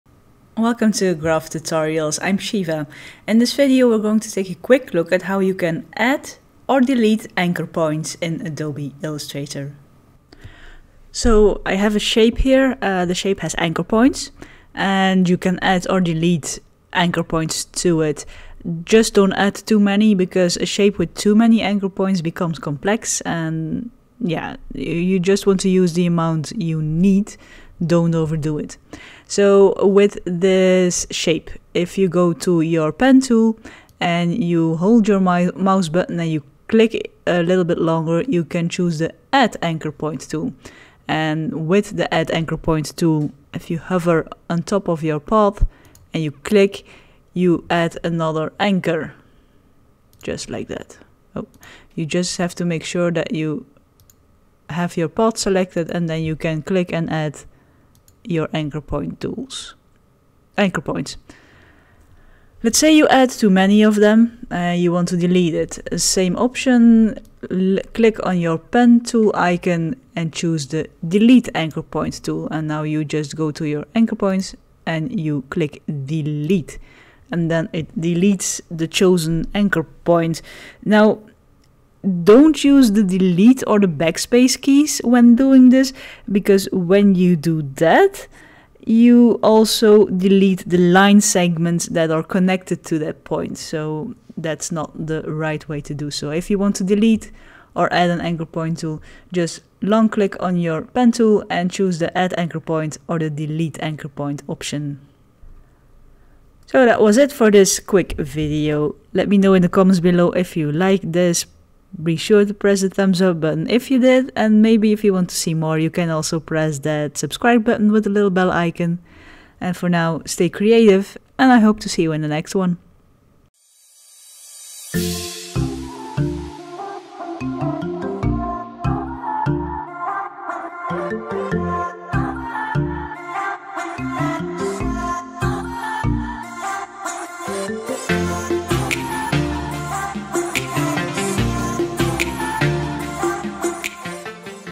How to add and delete anchor points [Adobe Illustrator]
A quick video on how you can add and delete anchor points in Adobe Illustrator.